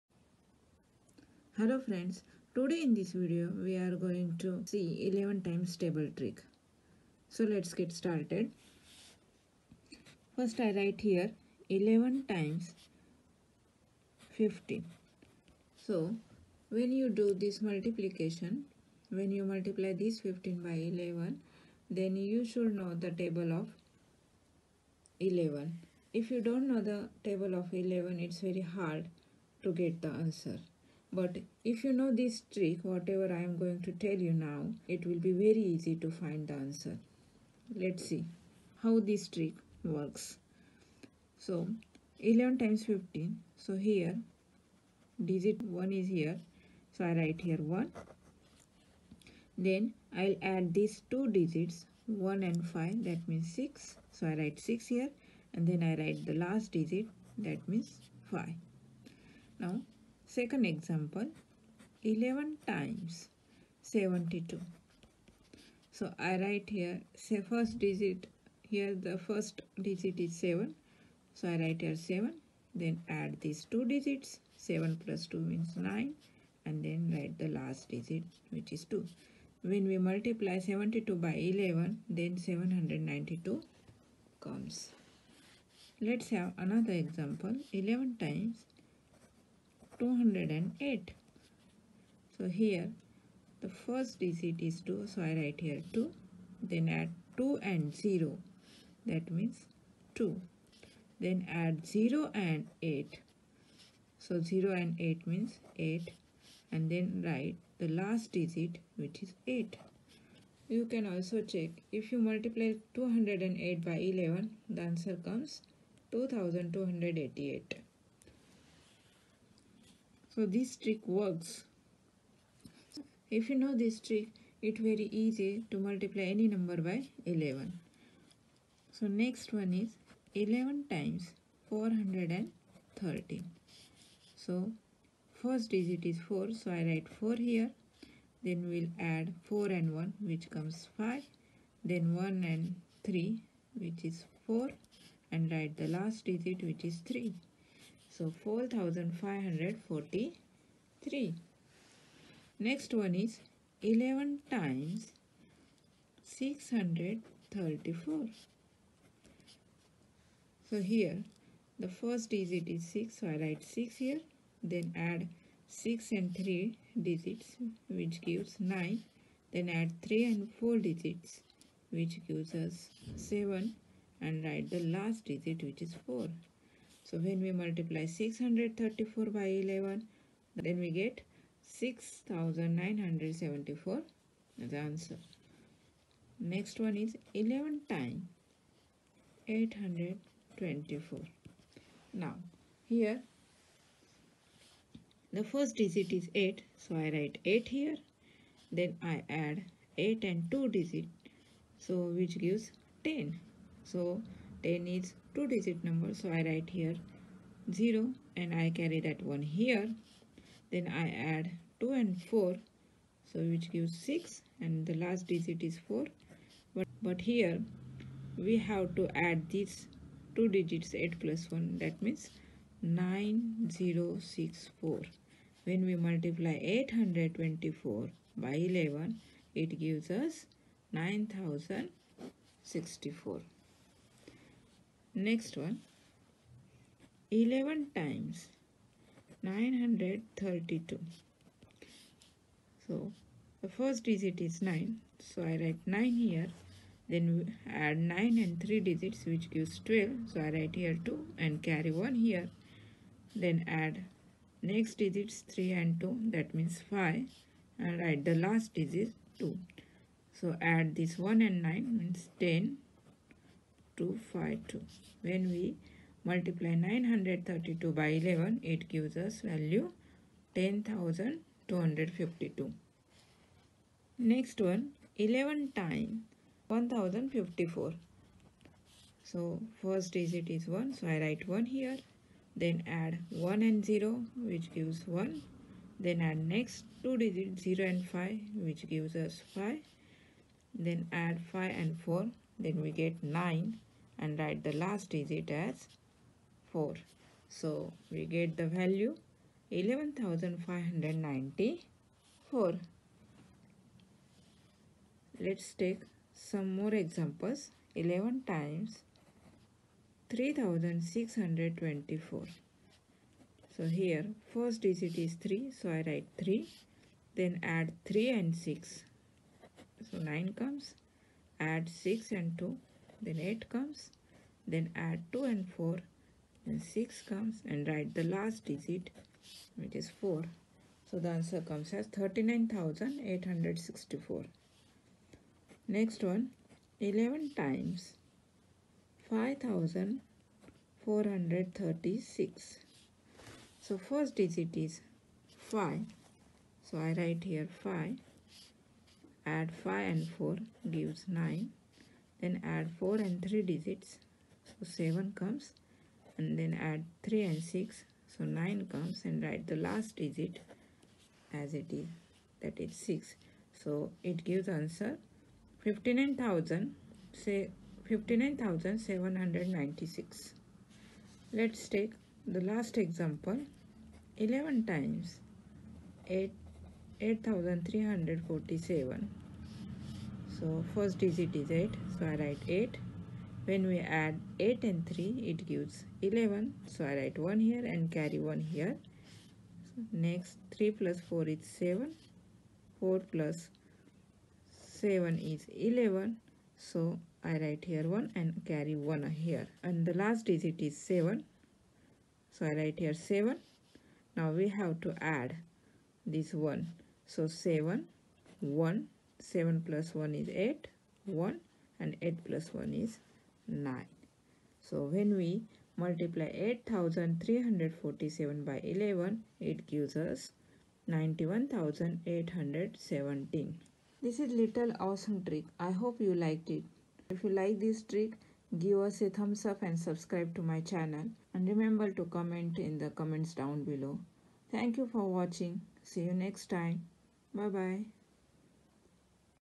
11 times table trick. Awesome trick. How to multiply any number by 11 within few seconds.
In this video 11 times , a table trick is shown. It is quite useful while solving multiplication by 11 numbers. If you follow this trick , you will save time and energy.